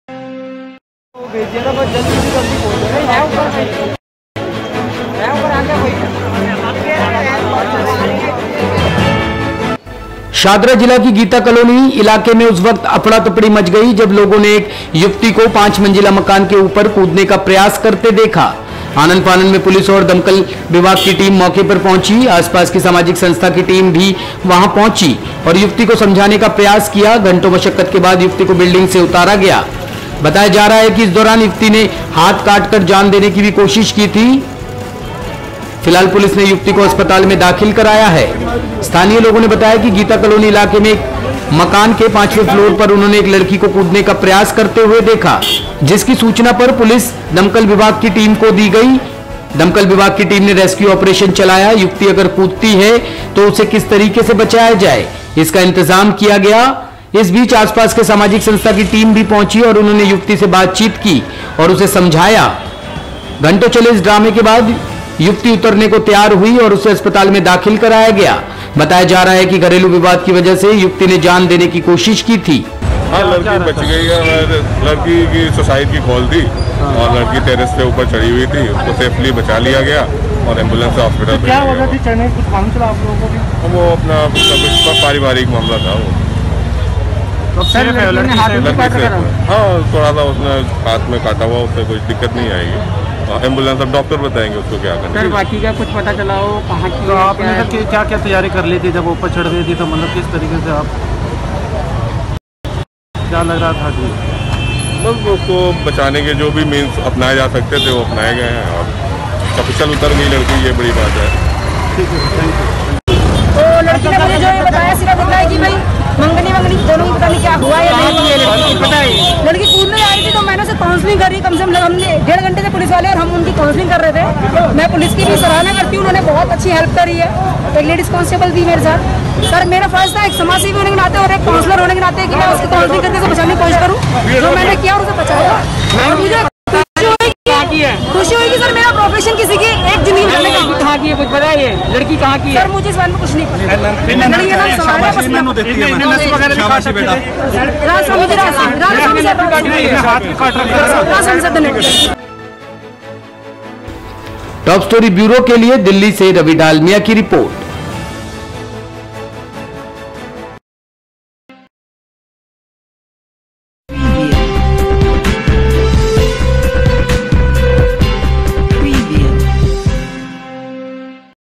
गीता कॉलोनी में एक युवती 5 मंजिला मकान से कूदने की कर रही थी कोशिश,पुलिस और लोगो ने बचाई जान
शाहदरा जिला की गीता कॉलोनी इलाके में उस वक्त अफरातफरी मच गई जब लोगों ने एक युवती को 5 मंजिला मकान के ऊपर कूदने का प्रयास करते देखा आनन-फानन में पुलिस और दमकल विभाग की टीम मौके पर पहुंची आसपास का सामाजिक संस्था की टीम भी वहां पहुंची और युवती को समझाने का प्रयास किया घंटों मशक्कत के बाद युवती को बिल्डिंग से उतारा गया बताया जा रहा है कि इस दौरान युवती ने हाथ काट कर जान देने की भी कोशिश की थी फिलहाल पुलिस ने युवती को अस्पताल में दाखिल कराया है।